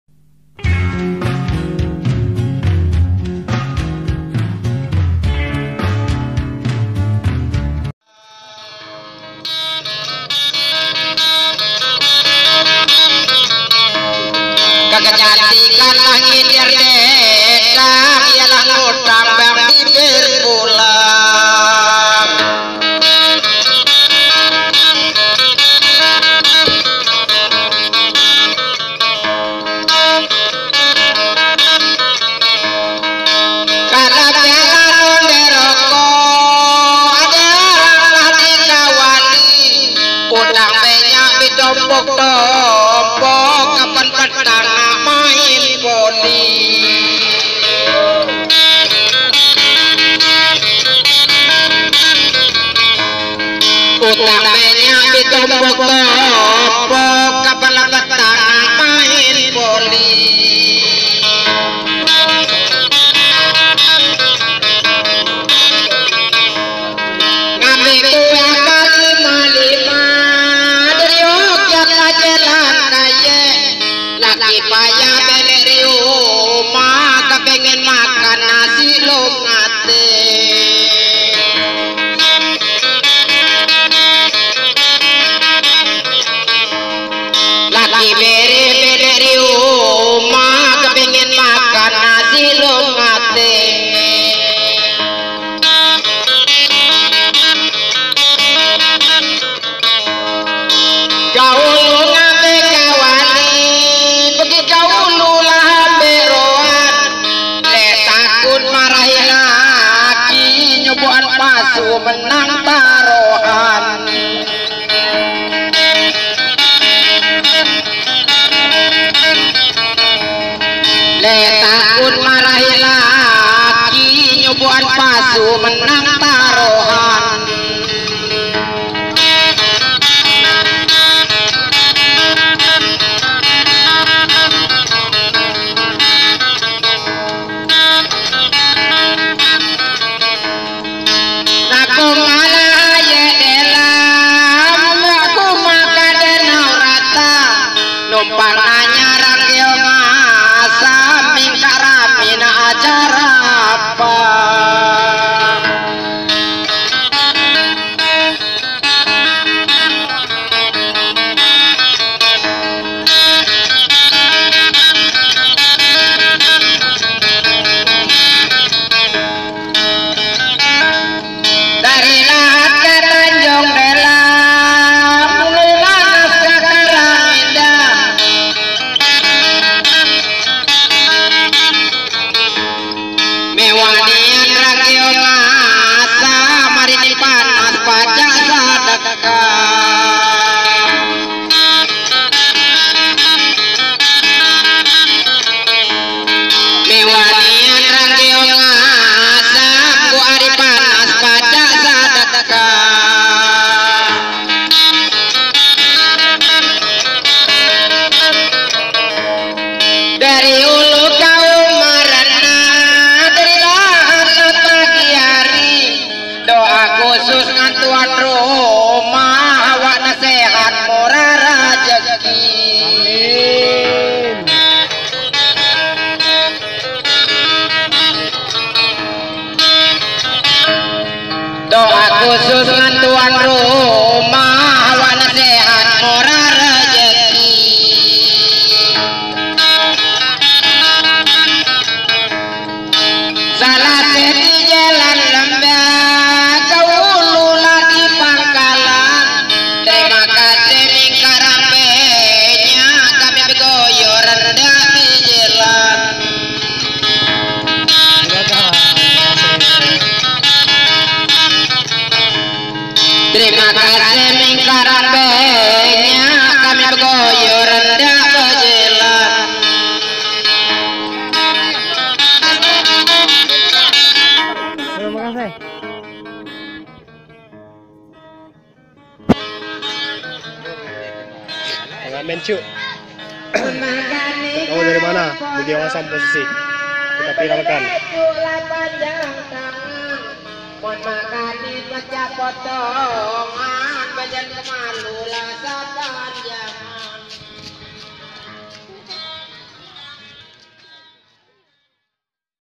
Tembang Belide Asli Mantul Pak
Seorang Bapak Buta Yang Tak Pernah Lelah Untuk Mencari Uang Demi Sesuap Nasi
Petikan Gitar Tembang Belide Asli
/Pengamen Jalan
/Asli Belide